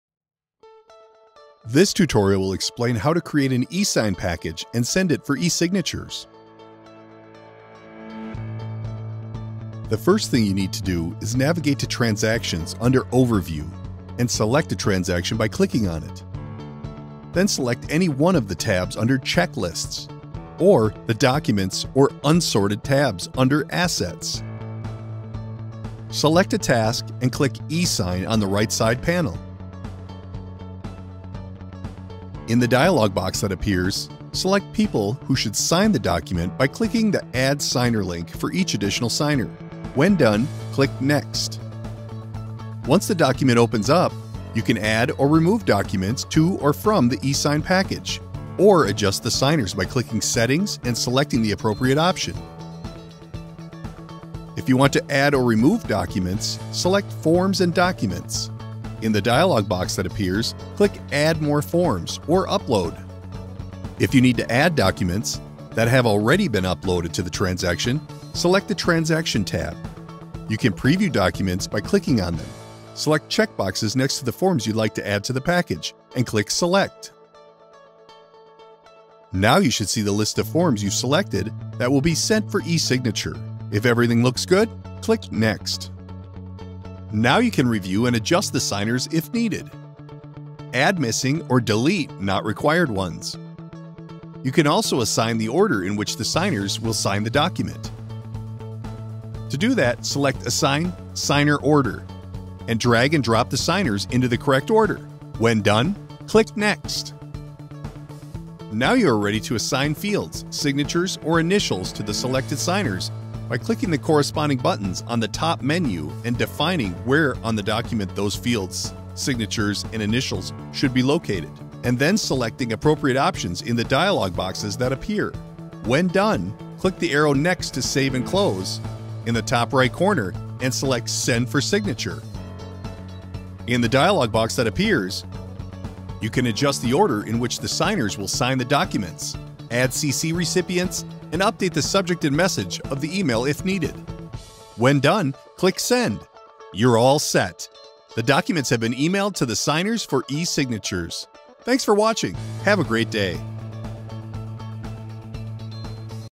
Creating an eSignature Package
Short video tutorial explaining how to create an eSignature package and send one or more documents for eSignature